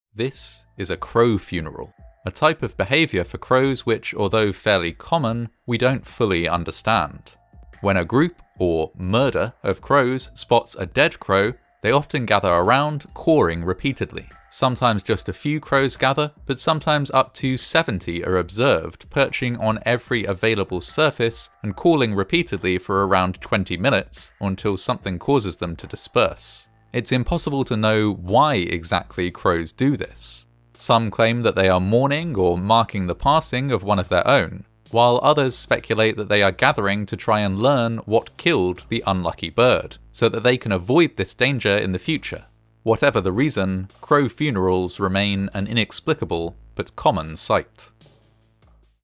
Crow Funerals | Fascinating Horror Shorts
Something quite strange happens when a murder of crows spots a dead one of their own...